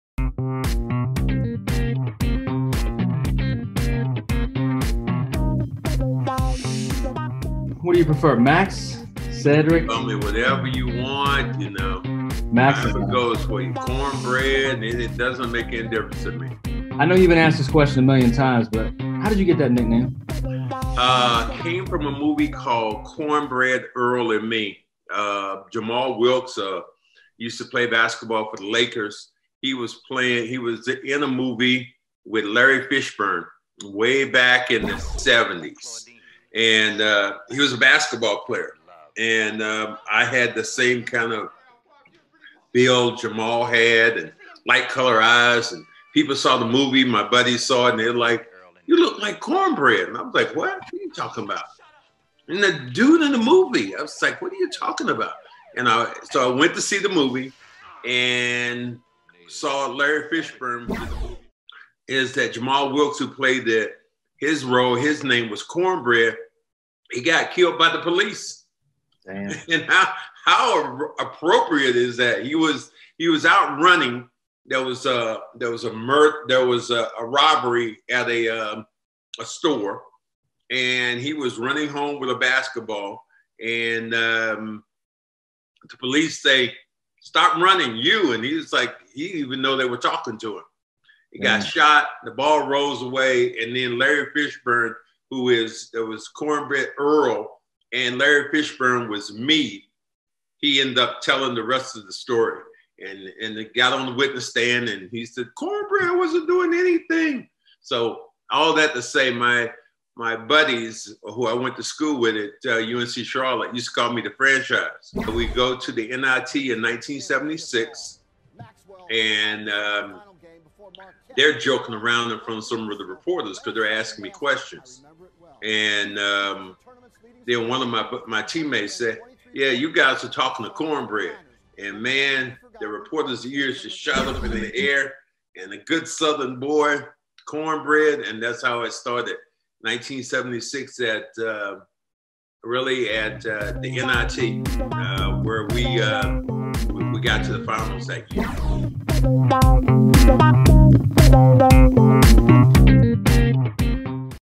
Cedric Maxwell on How ‘Max’ Became ‘Cornbread’ - Celtics Legend Finally Reveals
Support CLNS programming, powered by LinkedIn Jobs. to support the hard working reporters and show hosts at CLNS, and save 50 dollars on your first job listing.

One of the most frequently asked questions of Celtics fans is, “why did they call Max Cornbread?” Followed by, “doesn’t Max hate being called ‘Cornbread?’

Today Cedric Maxwell answers that question.

Joined by the filmmaker, Marcus Mizelle, Max hands over the interviewing reigns and allows Marcus to interview him.

Marcus Mizelle and Maxwell have a unique bond. Both are from the sleepy town of Kinston, NC. Additionally, Mizelle is the director of the documentary short Something in the Water: A Kinston Basketball Story, about all the extraordinary basketball talent the comes from Kinston. Cornbread, having led UNCC to an unlikely NIT championship runner up, and winning 2 NBA championships & a Finals MVP, is arguably the greatest Kinston native.

This segment is the first in a series produced from the 90 min, full audio version that will release on iTunes and Spotify, in November.